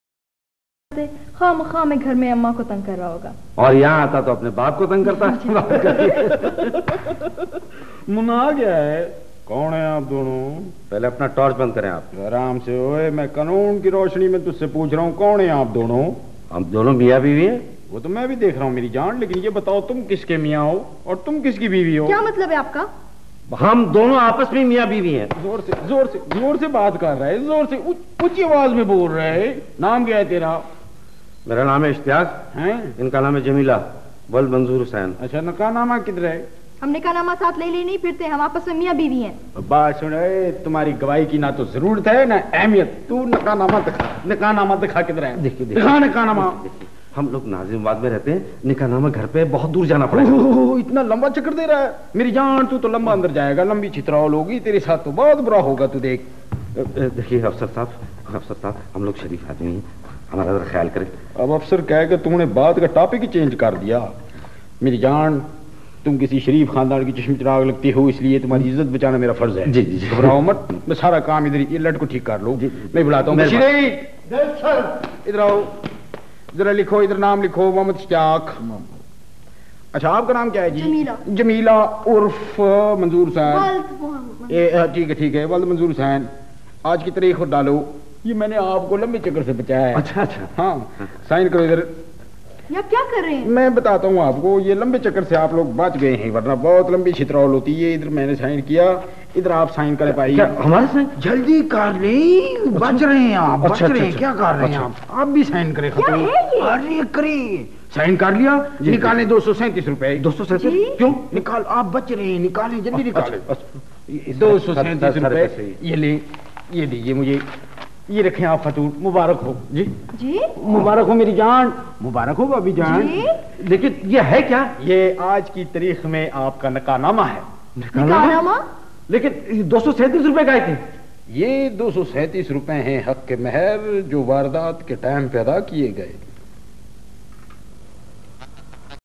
PTV old days memories
ptv old days memories comedy clip Legendary stage and television
Actors Moin Akhter and Latif Kapadia classic comedy clip original VHS
recording
ALL IN ONE